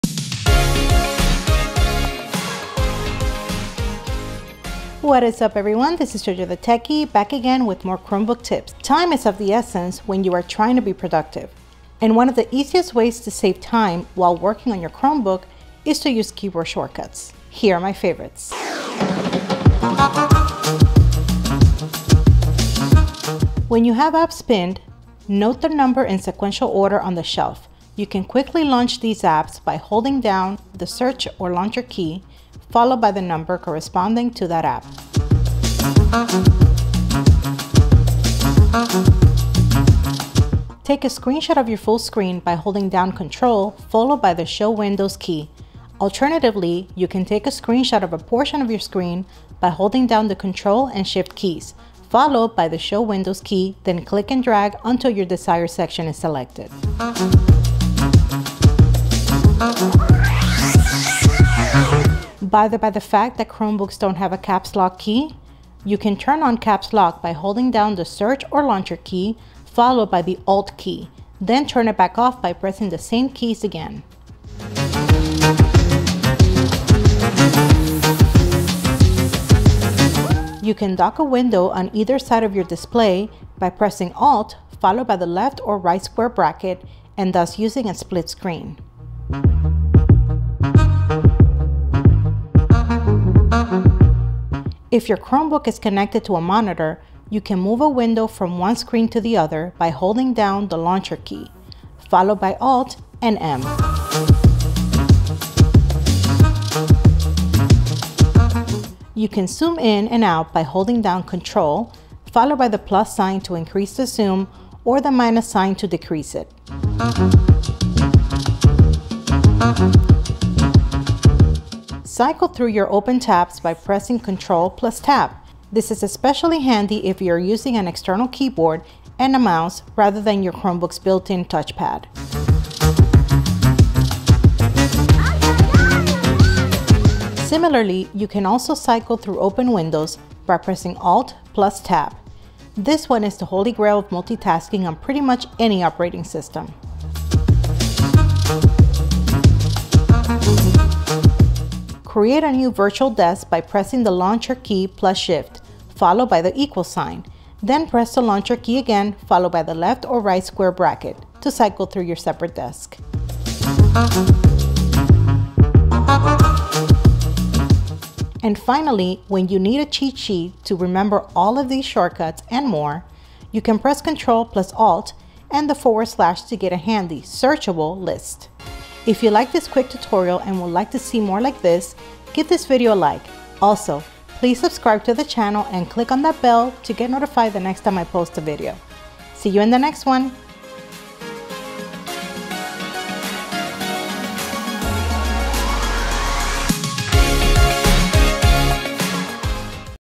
Best Chromebook Shortcuts to Boost your Productivity
There are many productivity hacks to make your Chrome OS experience as effortless as can be, but none more than chromebook shortcuts can take your productivity to a new level.

CHAPTERS
0:00 - Intro
0:19 - Launching Pinned Apps
0:35 - Taking Screenshots
0:57 - Caps Lock
1:15 - Docking Windows
1:30 - Moving Between Screens
1:44 - Zooming In and Out
1:57 - Cycle Through Open Tabs
2:12 - Cycle Through Open Windows
2:27 - Manage Virtual Desks
2:43 - Shortcut to all the Shortcuts

📷 My primary camera: Sony ZV-1
💻 My Laptop: Google Pixelbook Go